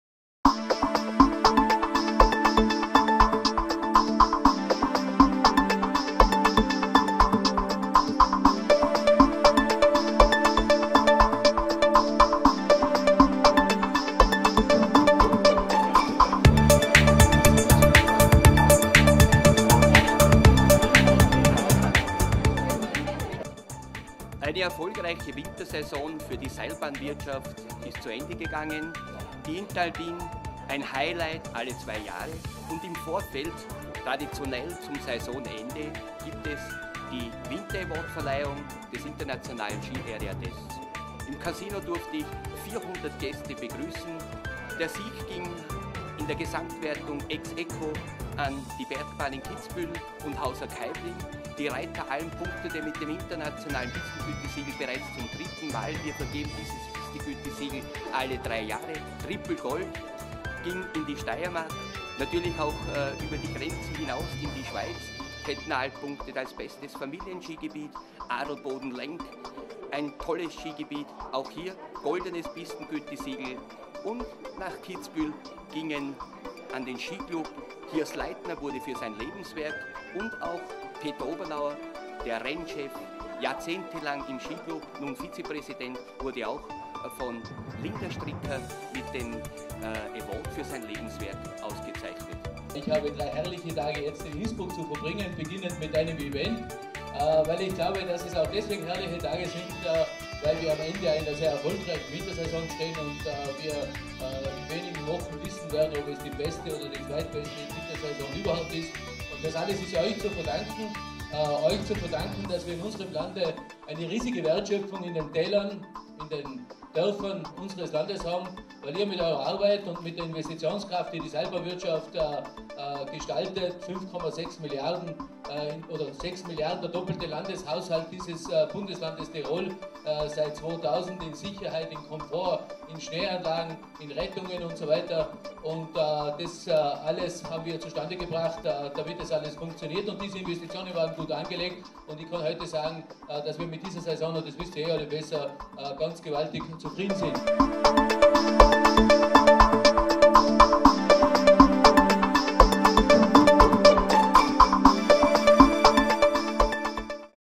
Interalpin.TV 2013 - Internationaler Skiareatest Winter-Awards 2012-13
Interalpin.TV @ Internationaler Skiarea Test Winter Award 2012-13 im eleganten Ambiente des Casino Innsbruck fand am 9. April 2013 die Verleihung der Winter-Awards und Vergabe der Pistengütesiegel statt. 400 Gäste aus der Seilbahn,- u. Tourismuswirtschaft u.a. NR u. Obmann des Fachverbandes der Seilbahnen Österreichs Franz Hörl, Kantonalsrat Leo Leker, LA Josef Geisler verfolgten mit Spannung die Auszeichnung der "Besten der Besten".

FIND US